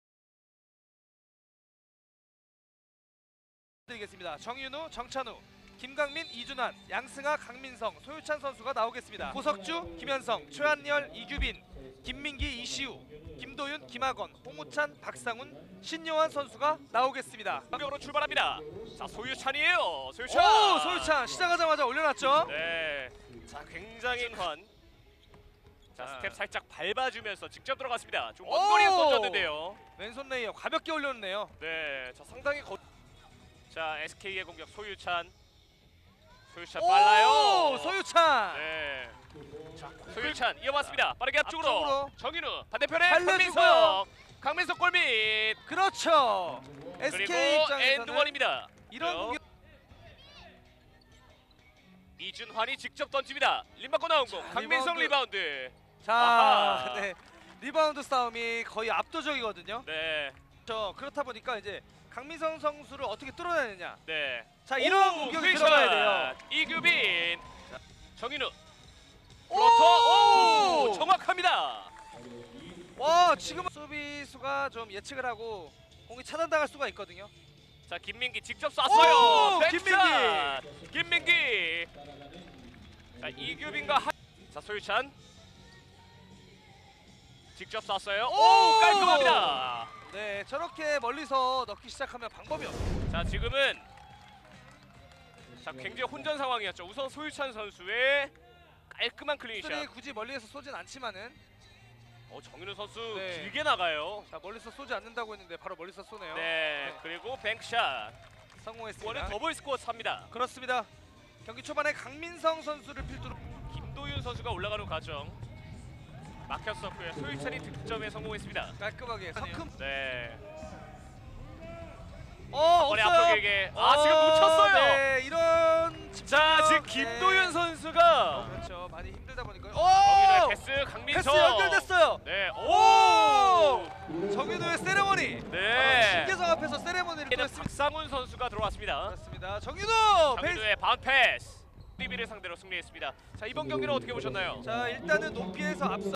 KBL유소년주말리그(2019.12.28) SK vs DB_U11 하이라이트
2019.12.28
원주종합체육관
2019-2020 KBL유소년주말리그 A권역
SK vs DB_U10